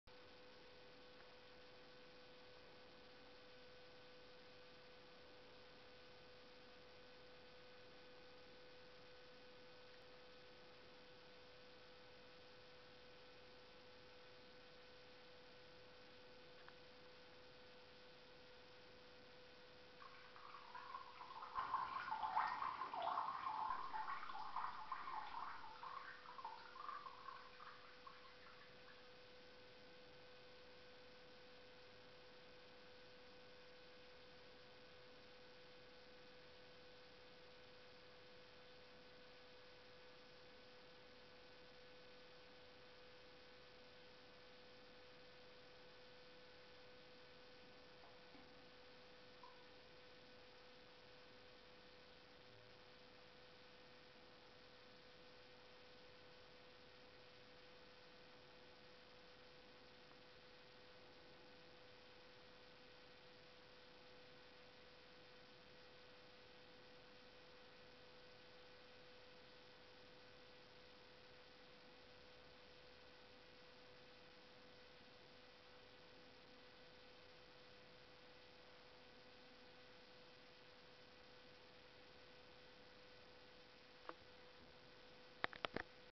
Gato faz xixi na privada
Meu animal de estimação fazendo sua necessidade na privada !